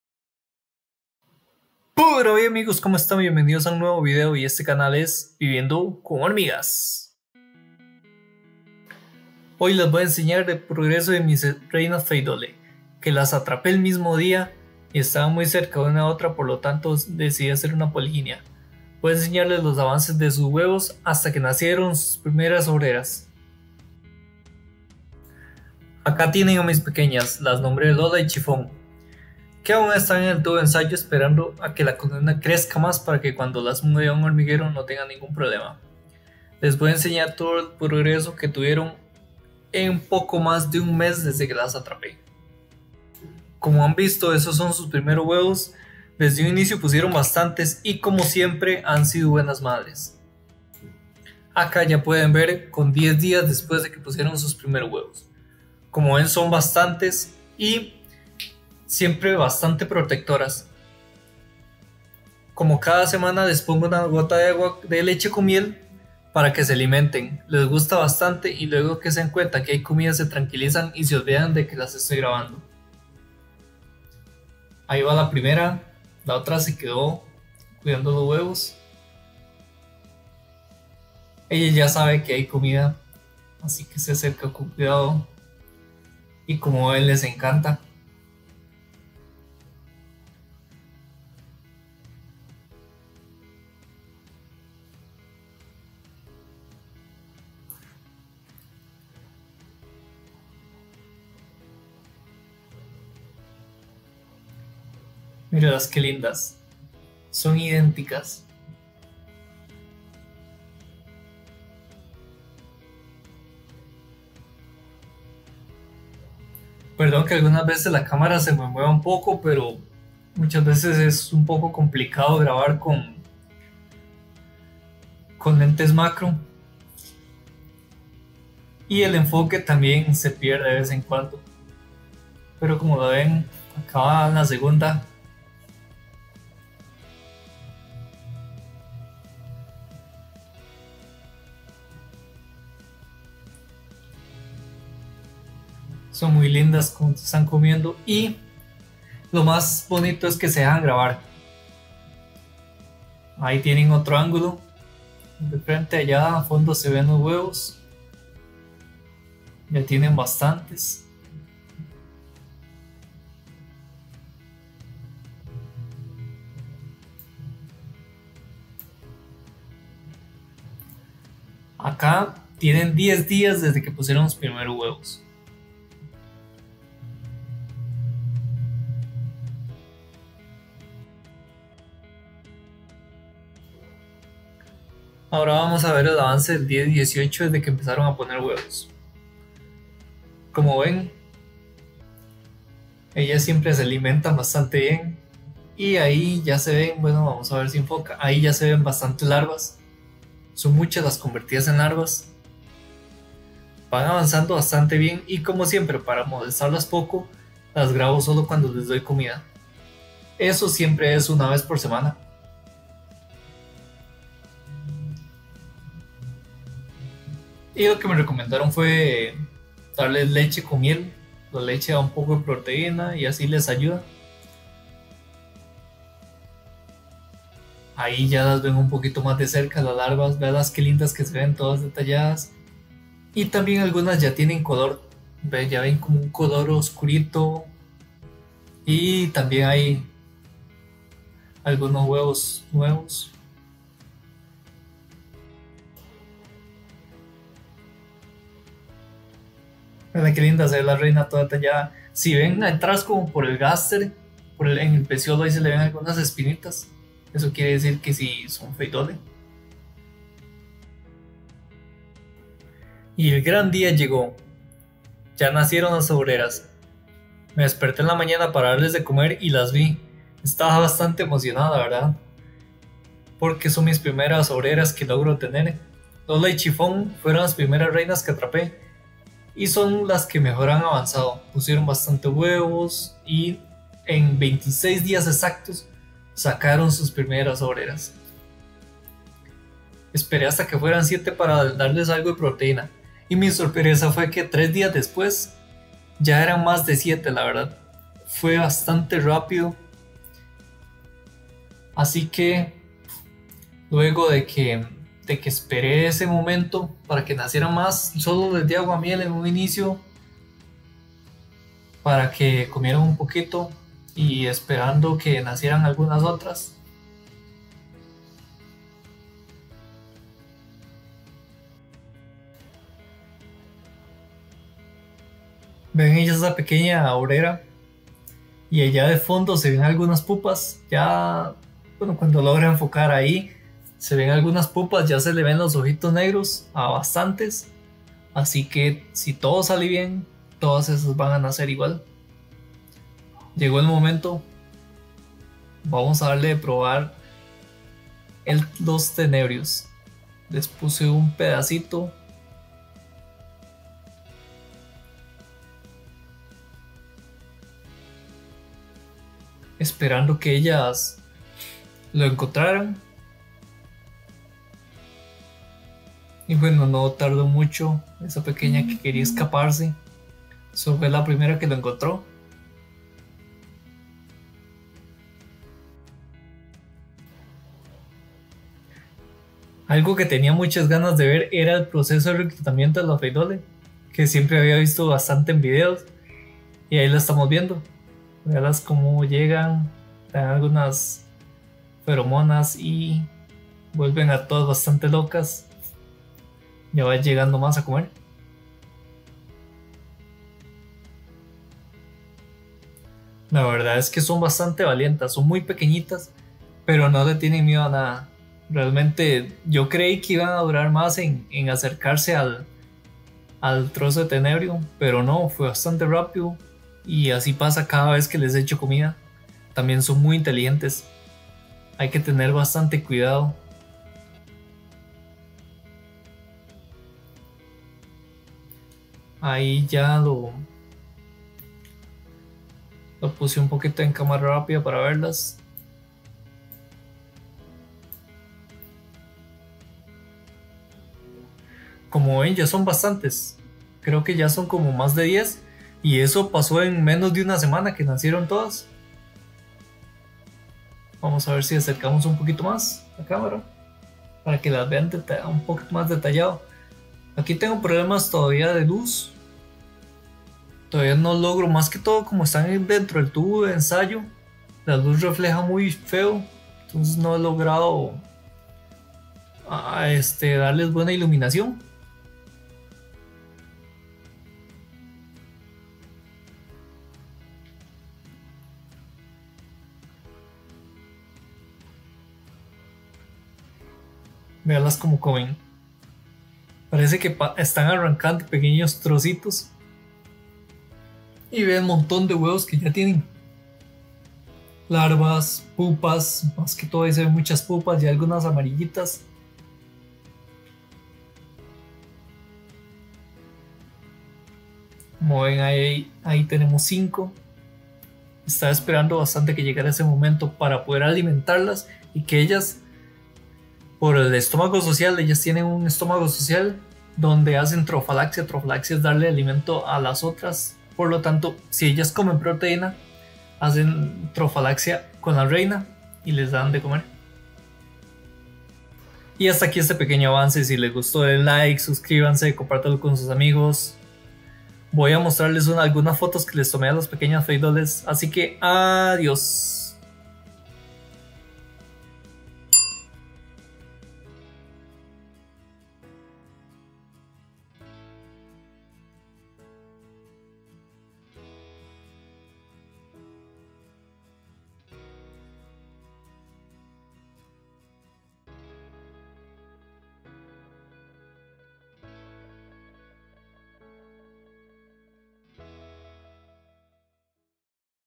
Progreso en el primer mes hormigas Pheidole sp. - Presentando a mi primera colonia de hormigas.
Les presento a mi mini colonia de hormigas Pheidole sp.

Les traigo el avance que tuvo en su primer mes en cada paso de mis dos reinas juntas en poliginia.

Me pueden seguir en mi instagram para ver mis colonias en macro